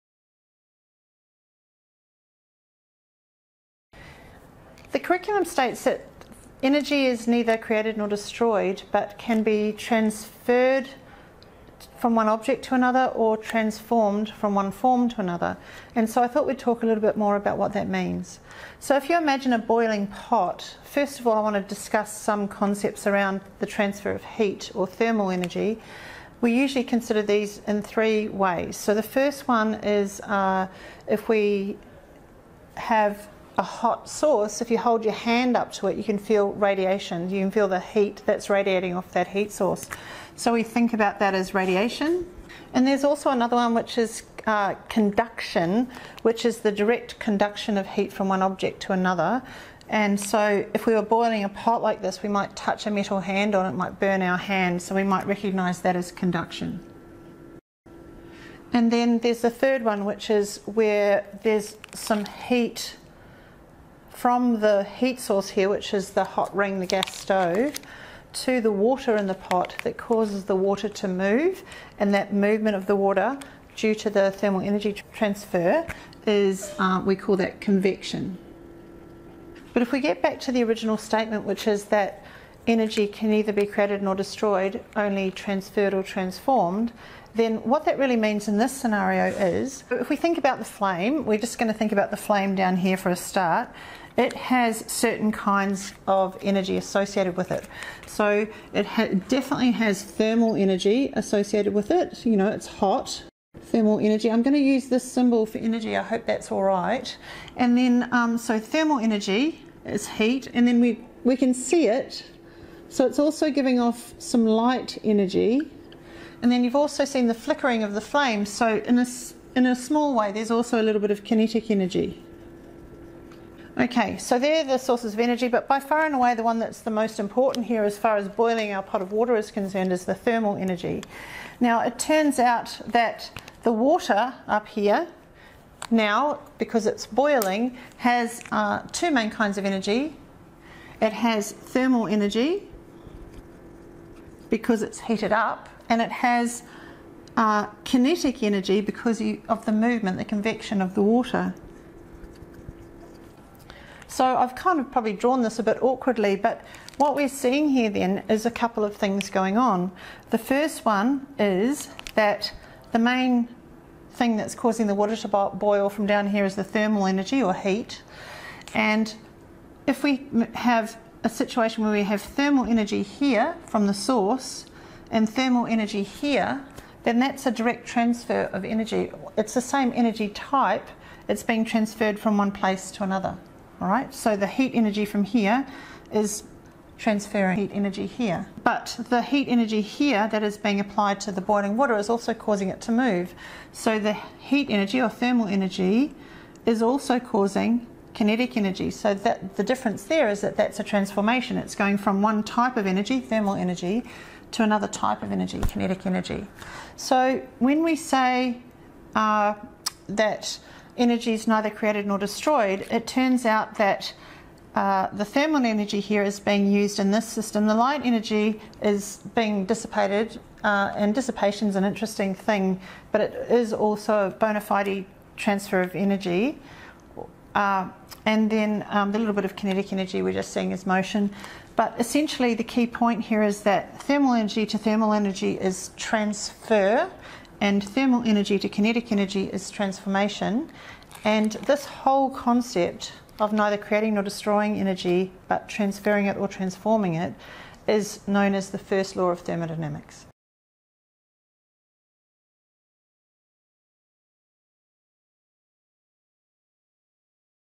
Transfers or transformations of energy | Earth Processes | meriSTEM
Associate Professor Leah Moore from the Australian National University (anu.edu.au) provides an overview of how energy is transferred and transformed, describing the first law of thermodynamics.

This is part of a series of educational videos on energy and Earth's systems. This clip has been created by the Research School of Earth Sciences (rses.anu.edu.au). These resources are part of a comprehensive program, curriculum matched to the unit 'Earth processes – energy transfers and transformations.'

This video touches on the following curriculum descriptors:
Energy is neither created nor destroyed, but can be transformed from one form to another (for example, kinetic, gravitational, thermal, light) and transferred between objects (ACSES044)